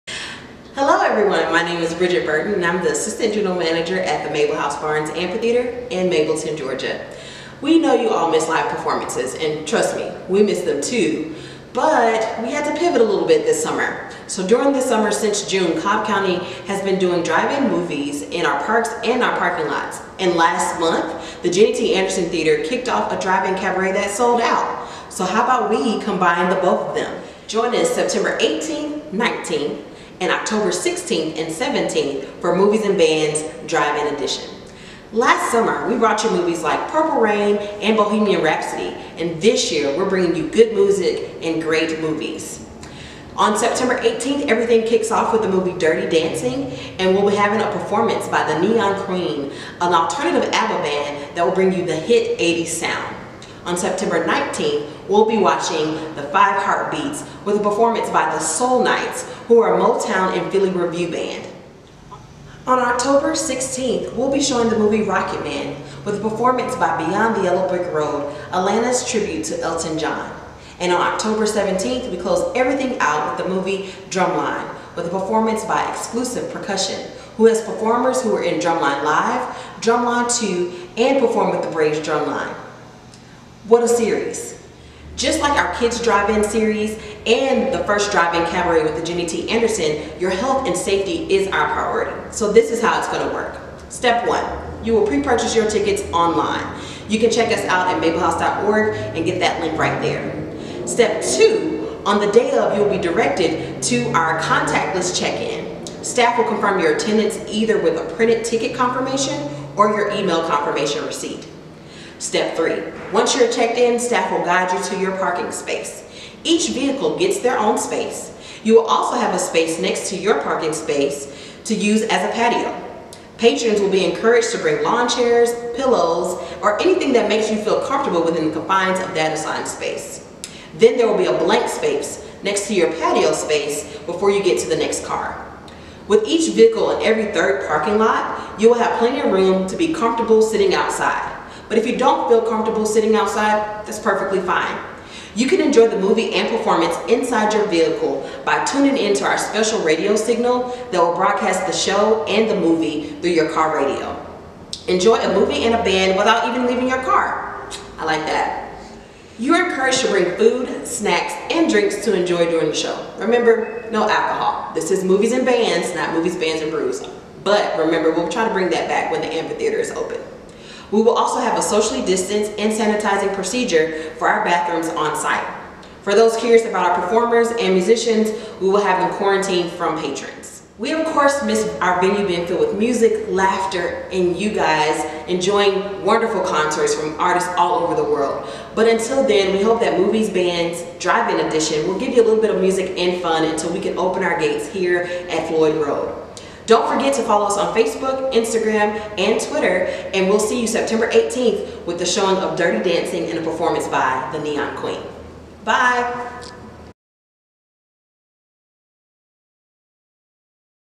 Mable House Barnes Amphitheatre Movies and Bands: Drive-In Edition 2020
Mable House Barnes Amphitheatre staff is excited to host its fall programming for movie and music lovers: Movies and Bands: Drive-In Edition. To best accommodate health and social distancing protocols, all movies and performances will be held at Al Bishop Park in Marietta. This series first launched last year as Movies, Bands & Brews and featured local acts performing songs from popular movies. Attendance soared in July 2019 with the screening of the movie “Purple Rain” and performance by Decatur rock band Hero the Band.

This year, the series will be a drive-in and showcase new musical acts and movies. This fun event kicks off the third weekend in September with two features: Friday, Sept. 18, there will be a showing of “Dirty Dancing” with a performance by The Neon Queen, an alternative Abba band, and on Saturday, Sept. 19, a showing of “The Five Heartbeats” with a performance by The Soul Knights, a Motown and Philly Revue act.

In October, the series culminates with a showing of “Rocketman” on Oct. 16, with a performance by Beyond the Yellow Brick Road Atlanta's Tribute to Elton John and a showing of “Drumline” with a performance by Xclusive Percussion on Oct. 17. This drumline entertainment company has members who are a part of Drumline Live, the Braves drumline and were in the movie “Drumline 2.”

All performances will start at 7 p.m. and the movies will begin at 8 p.m. Tickets are $50 per vehicle and go on sale at 10 a.m., Thursday, Sept. 3. Patrons can purchase tickets through Civic Rec